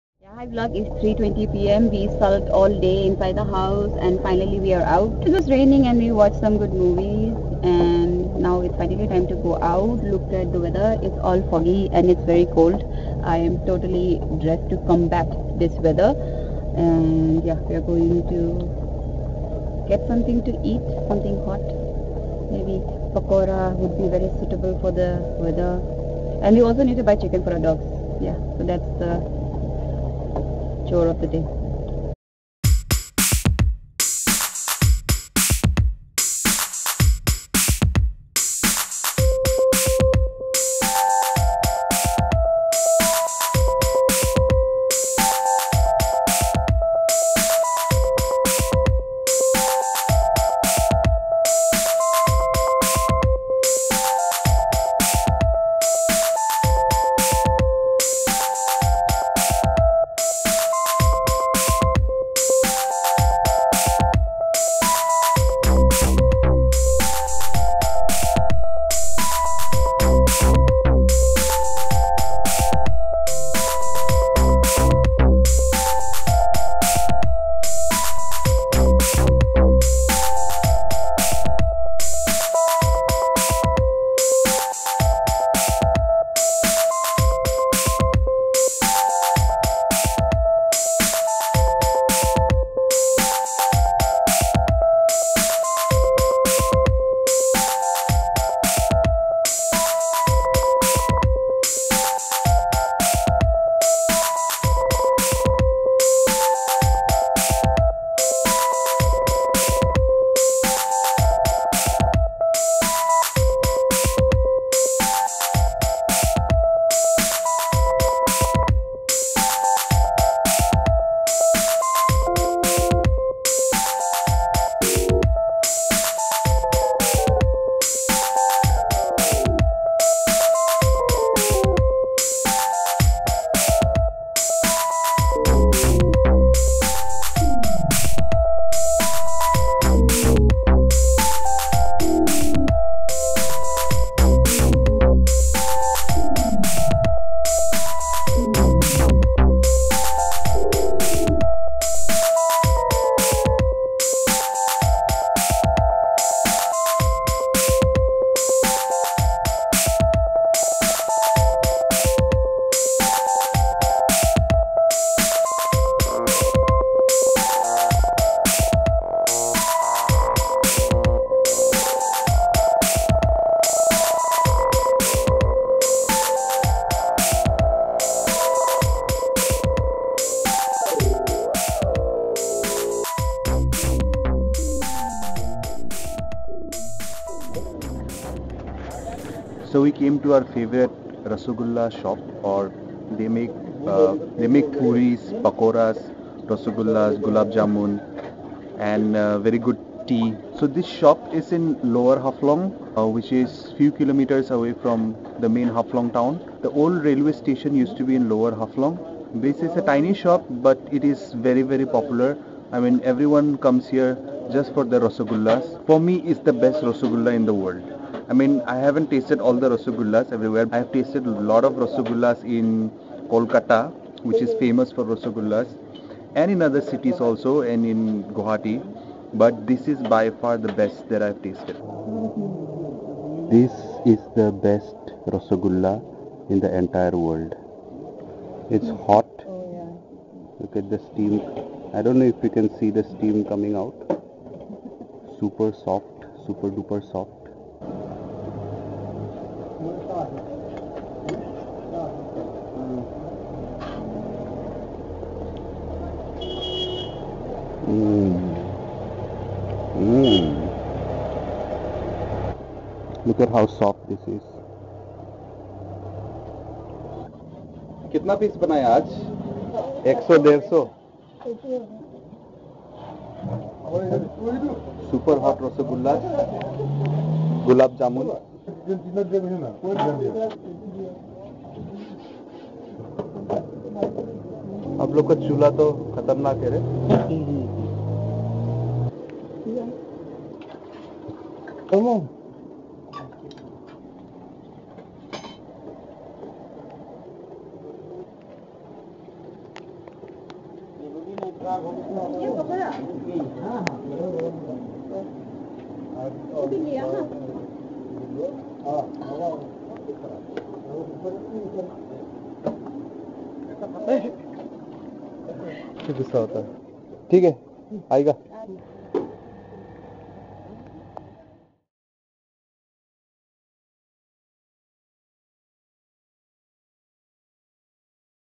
the BEST RASGULLA!
A small tea shop in Lower Haflong sells the best rasgulla you can ever find.